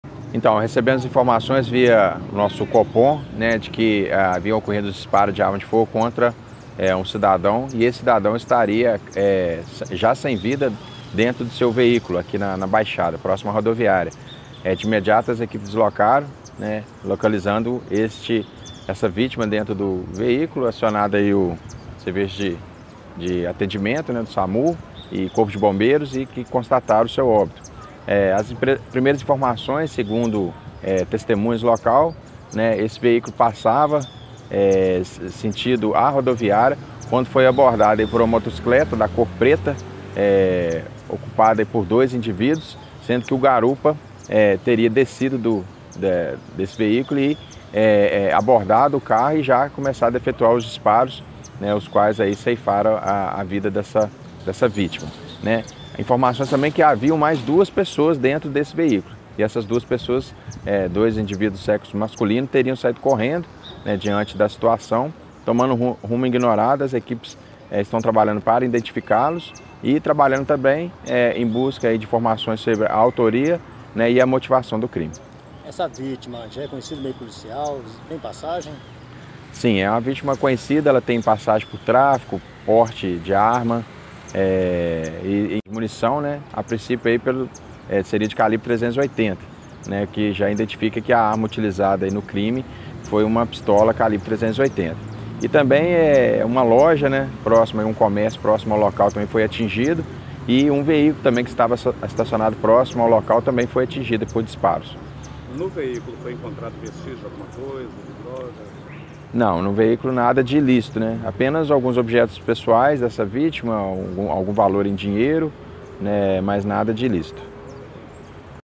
Rapaz de 25 anos é executado a tiros dentro do carro na Baixada, em Manhuaçu
Luís Octávio Fernandes, 25 anos, foi assassinado a tiros dentro de seu carro na esquina das Avenidas Barão do Rio Branco e Salime Nacif, na Baixada, em Manhuaçu, por volta de 6 horas deste sábado, 10/02.

Segundo as informações, dois homens numa motocicleta preta se aproximaram do Celta e o garupa atirou várias vezes. O carro parou na calçada da praça José Adolfo Assad (praça do skate). Após o crime, os autores fugiram.

Equipes do Corpo de Bombeiros e SAMU foram acionadas. O jovem morreu no local.

A perícia da Polícia Civil recolheu 13 estojos de munição calibre 380.

Segundo a PM, Luís Octávio tinha diversas passagens por envolvimento com tráfico de drogas. Ele teria saído de casa, no bairro São Francisco de Assis, e iria buscar recuperandos da APAC e leva-los para o trabalho.

Segundo o Tenente Vieira, havia dois homens no veículo e eles saíram correndo. A PM tenta localizar os dois, bem como identificar e localizar os autores e a motivação do crime.